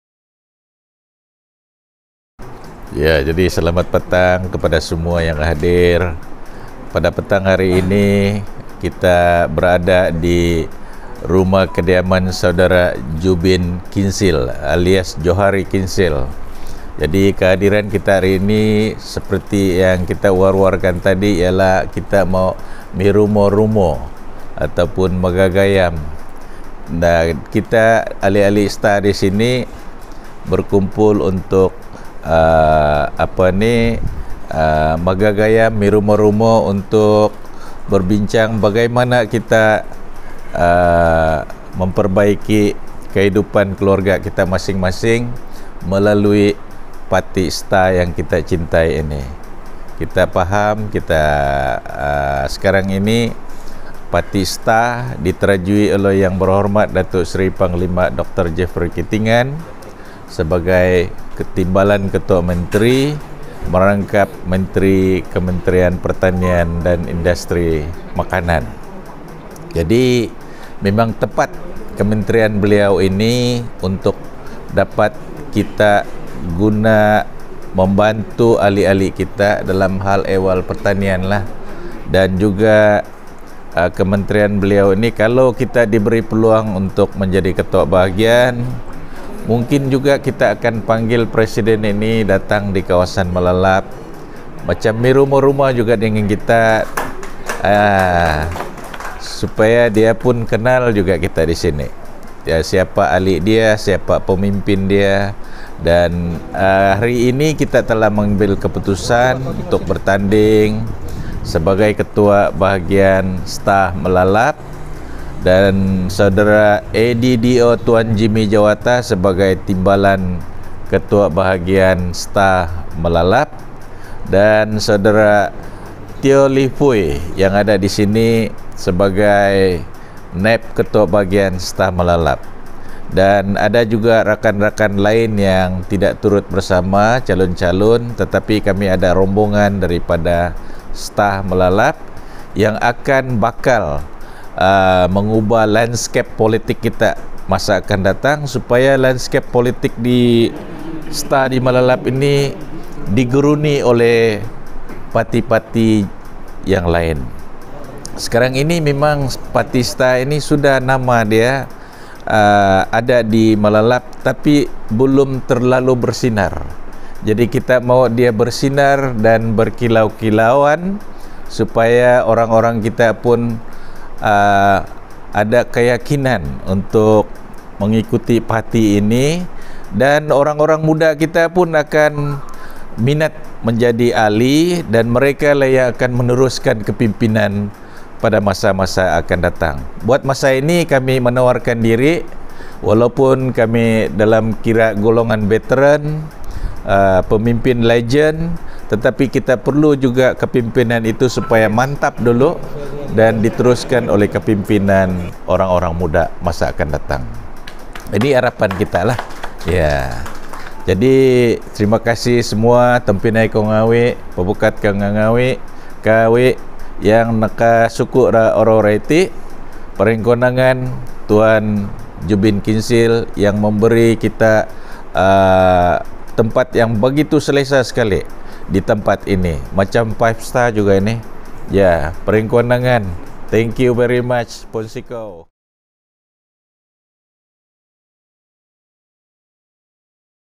Mirumo-rumo bersama Datuk Dr. Lucas Umbul dan ahli STAR Kg. Makaniton.
Mirumo-rumo bersama Datuk Dr. Lucas Umbul serta rombongan AJK bahagian STAR N.42 Melalap.di Kg.Makaniton.